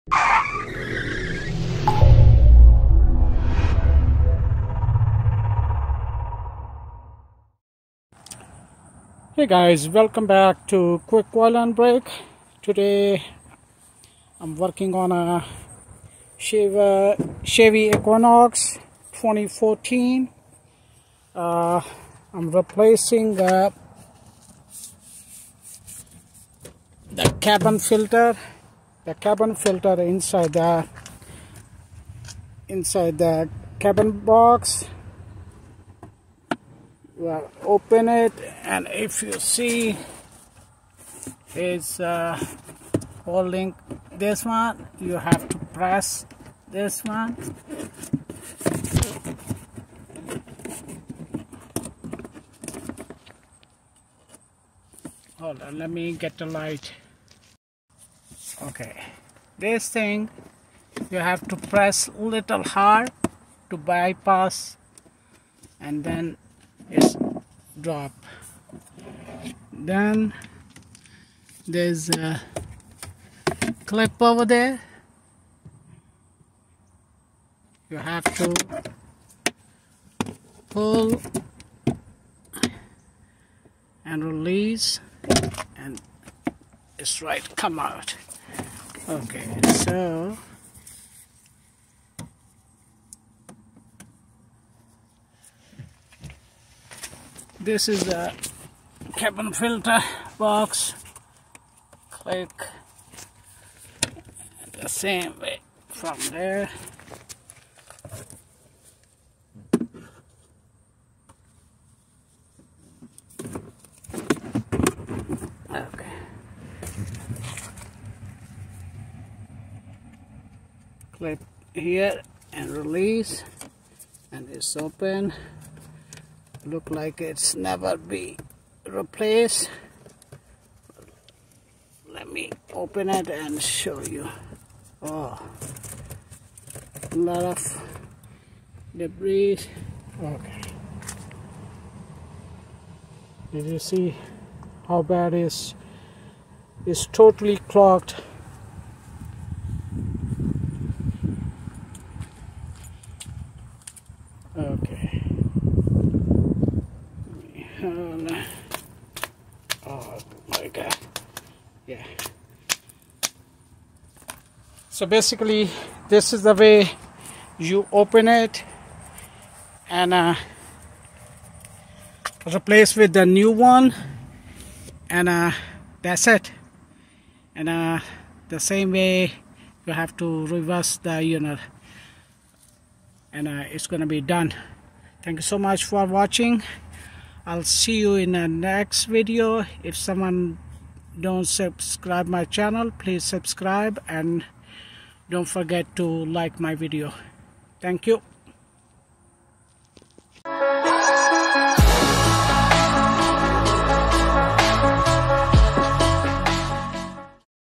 How to replace Cabin Air Filter on 2014 Chevy Equinox.
Check this out before replacing your cabin air filter.